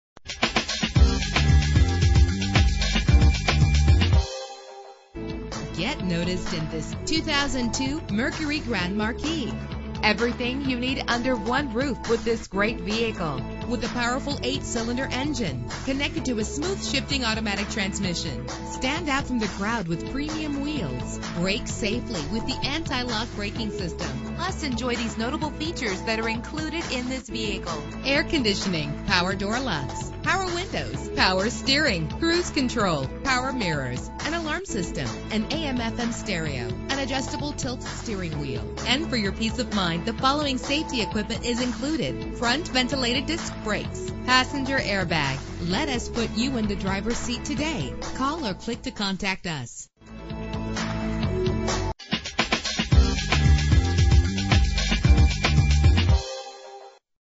2002 Mercury Grand Marquis - Huntersville NC
Year: 2002
Make: Mercury
Model: Grand Marquis
Trim: LS
Engine: 4.6 liter 8 cylinder 16 valve
Transmission: 4-Speed Automatic
Color: White
Mileage: 120335
Address:
13825 Statesville Rd